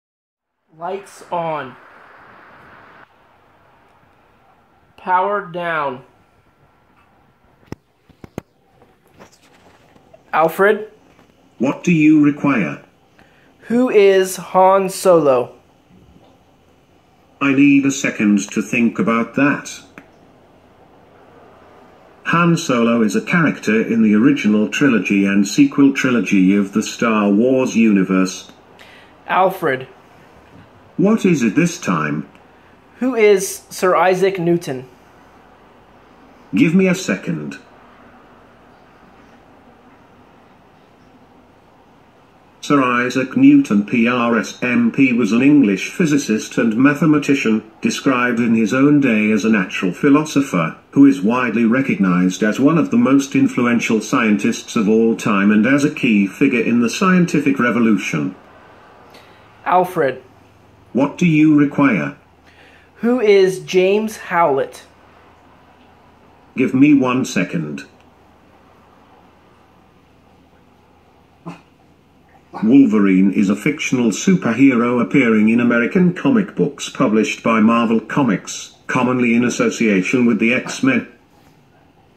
ALFRID Mark 2 test
A test of my artificial intelligence platform which was inspired by Iron Man's JARVIS. This version features a voice user interface and a question and answer artificial intelligence. This is just the start and it will get much more advanced in the near future. If you are interested in purchasing one to sign up for a notification when they become available.

Special thanks to Mach_5 for the temporary case design.